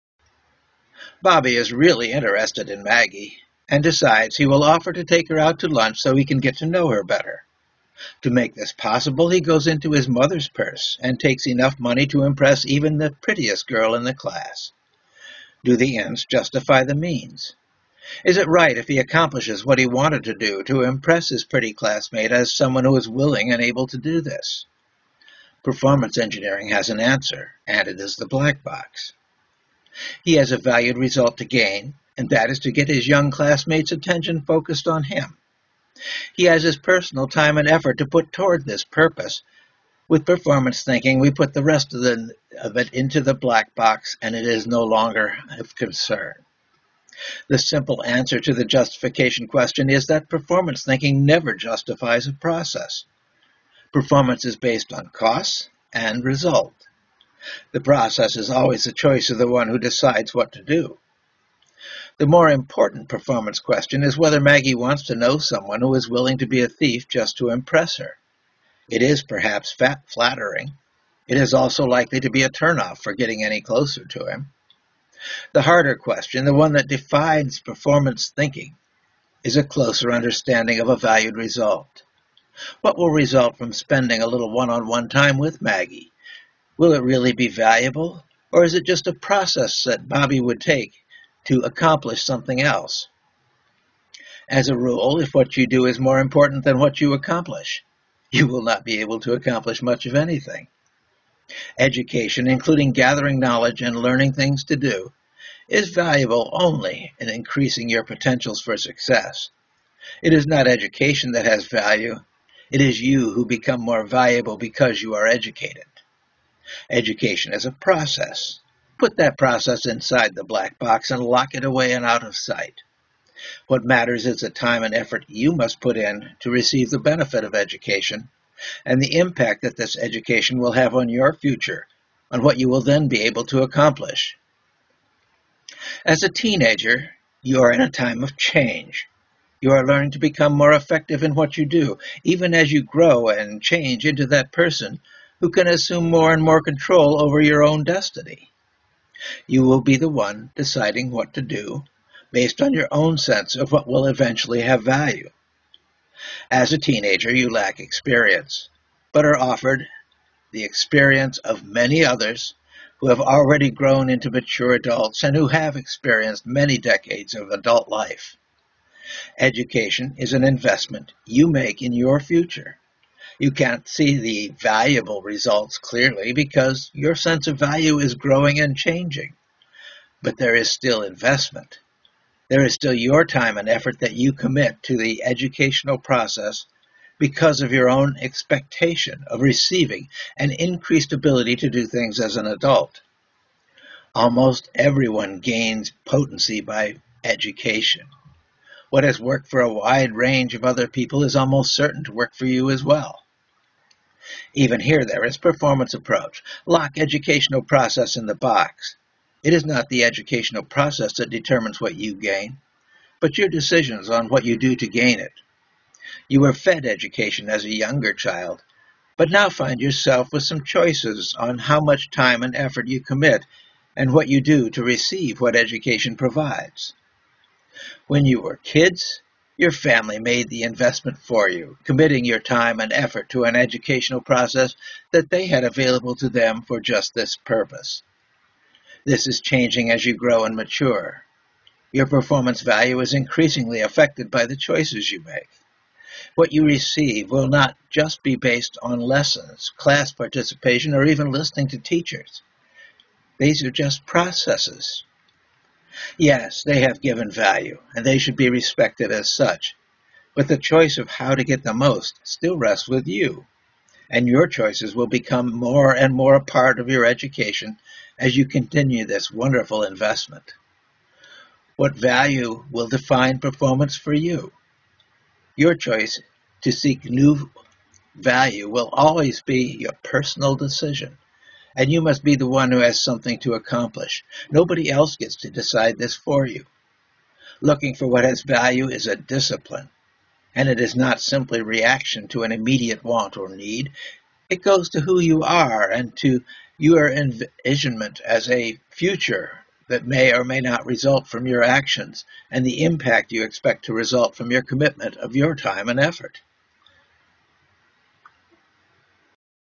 Unizor - Performance and Education
Unizor - Performance & Education
by Jesse Brogan

As a teenage student, it really is
all about you.
It is not the teachers, or the school, or even your parents that provide purpose.

Our first blackbox application presents technical support for your intelligent investment in education.
It examines your purpose in assuring your future with what you choose to do as your part in the educational effort.